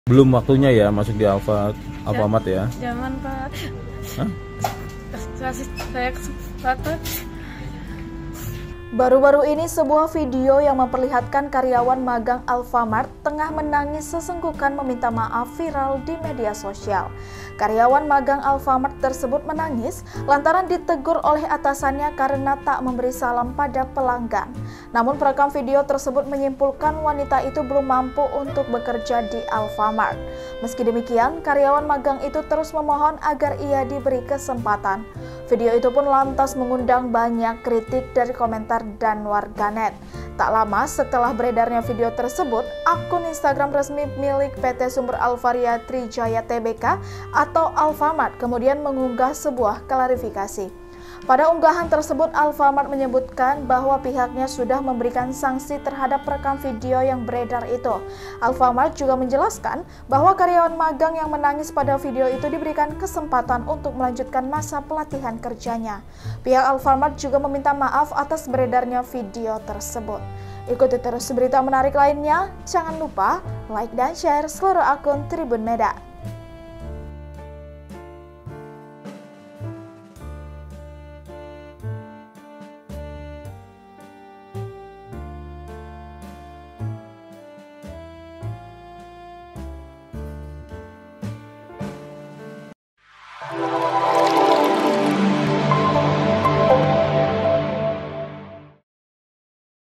MIRIS Pegawai Nangis dan Terancam Dipecat Gegara Tak Beri Salam, Begini Klarifikasi Alfamart
Karyawan magang Alfamart tersebut menangis lantaran ditegur oleh atasannya karena tak memberi salam pada pelanggan.

Namun perekam video tersebut menyimpulkan wanita itu belum mampu untuk bekerja di Alfamart.

Meski demkiian, karyawan magang itu terus memohon agar ia diberi kesempatan.

Video itu pun lantas mengundang banyak kritik dan komentar dari warganet.

Tak lama setelah beredarnya video tersebut, akun Instagram resmi milik PT Sumber Alfaria Trijaya Tbk atau Alfamart kemudian mengunggah sebuah klarifikasi.

Pada unggahan tersebut, Alfamart menyebutkan bahwa pihaknya sudah memberikan sanksi terhadap karyawan yang merekam video yang beredar itu.

Lebih lanjut, Alfamart juga menjelaskan bahwa karyawan magang yang terdapat dalam video itu diberikan kesempatan untuk melanjutkan masa pelatihan kerjanya.

Pihak Alfamart juga meminta maaf atas beredarnya video calon karyawannya yang menangis karena ditegur atasannya itu.